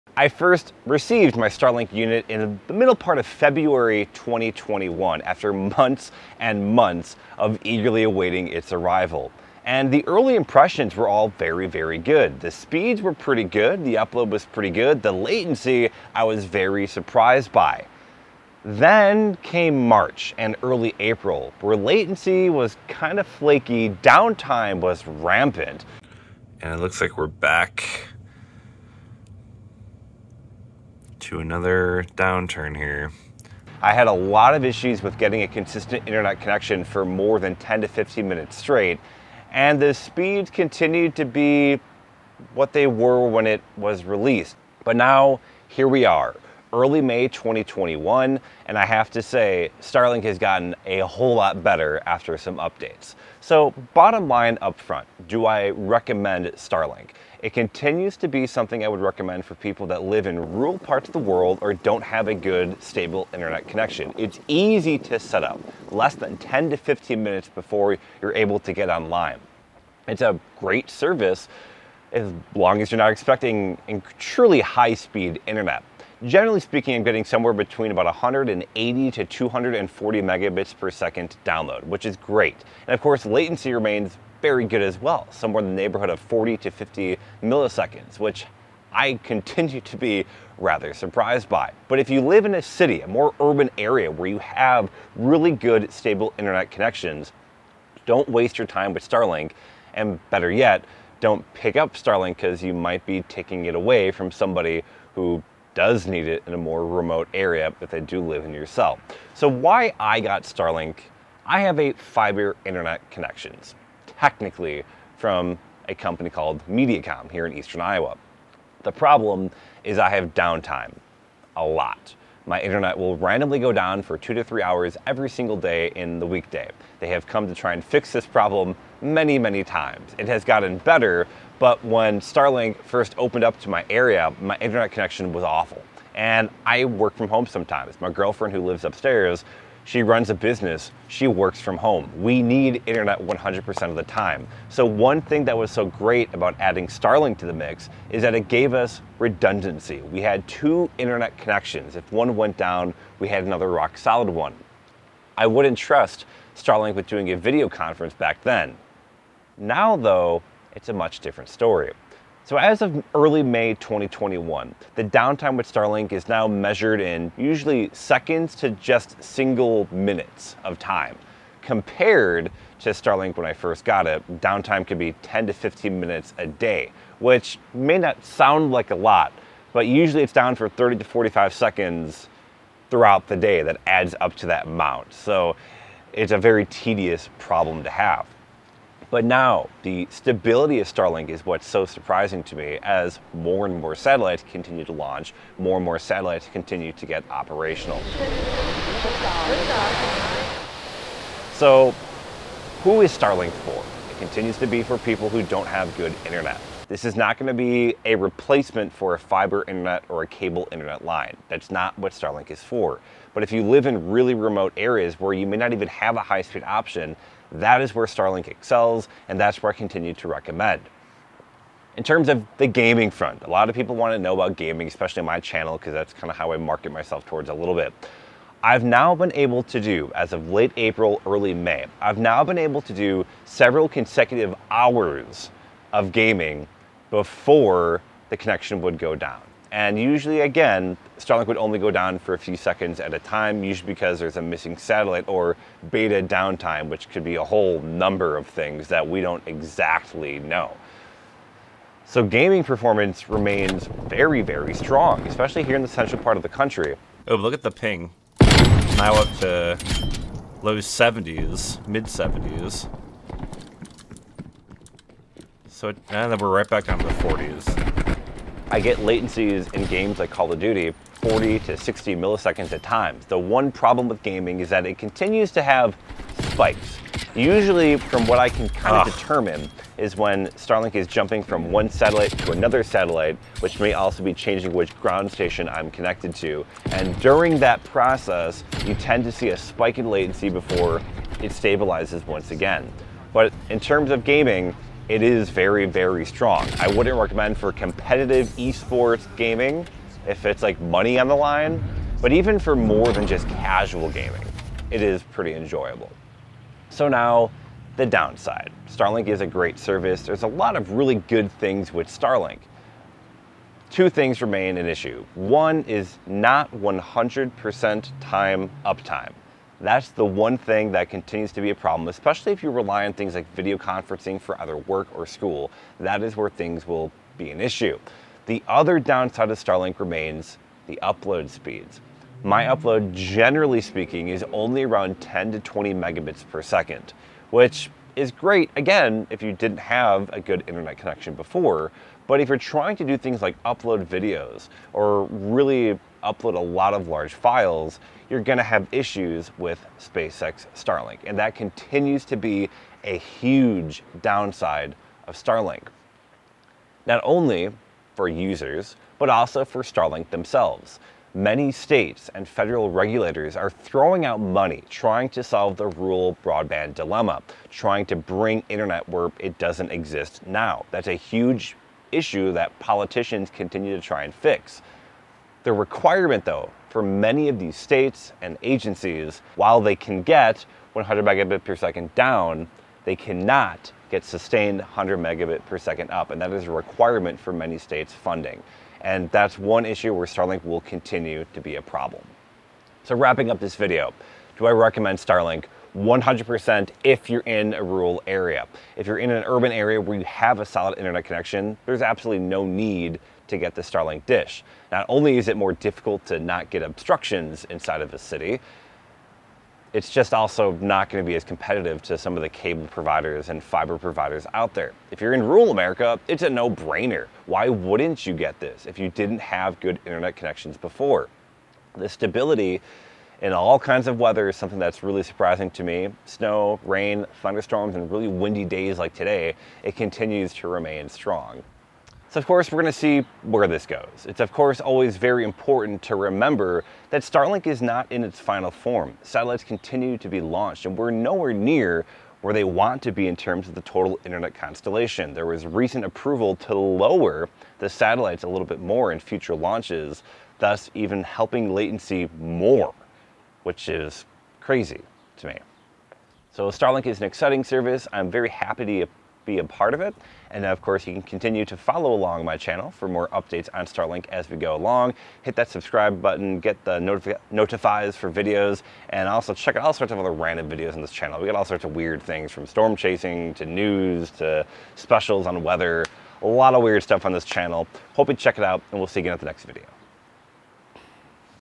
STARLINK May 2021 - Is it worth getting?
Join Robinhood with my link and we'll both get free stock 🤝

Between improving download speeds and rock-solid ping, especially for it being satellite internet, SpaceX Starlink remains a disruptive force. It has a lot to love, but has some catches as well...

0:00 - Introduction
02:38 - May 2021 Update
03:40 - Gaming Performance
05:06 - The Bad of Starlink